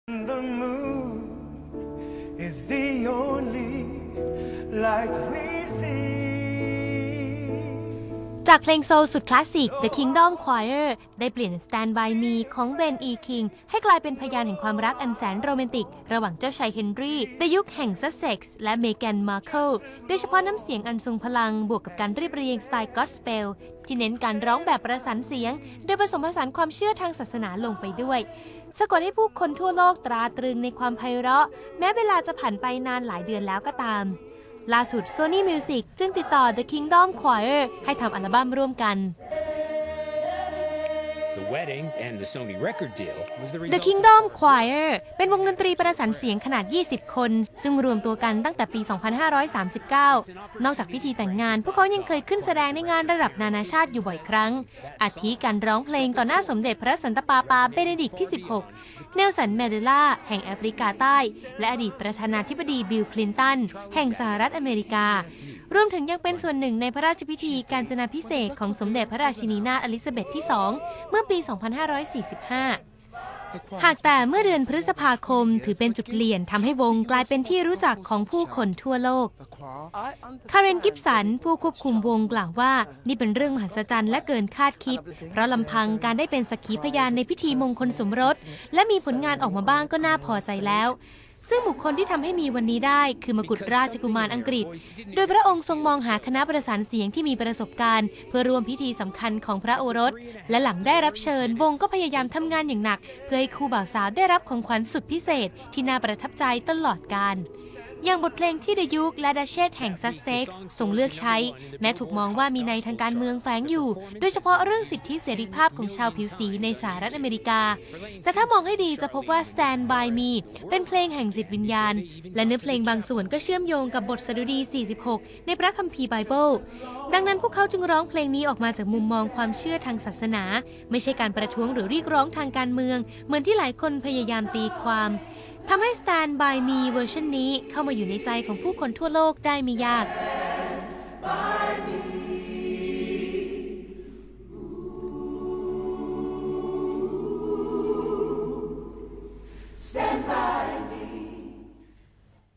วงประสานเสียงในพิธีเสกสมรสเจ้าชาย Henry เตรียมออกอัลบั้มเพลง
กลุ่มนักร้องประสานเสียงผิวสีที่เคยสร้างความประทับใจในพิธีเสกสมรสของเจ้าชาย Henry แห่งราชวงศ์อังกฤษ เตรียมออกผลงานเพลงของตัวเองในเดือนพฤศจิกายนนี้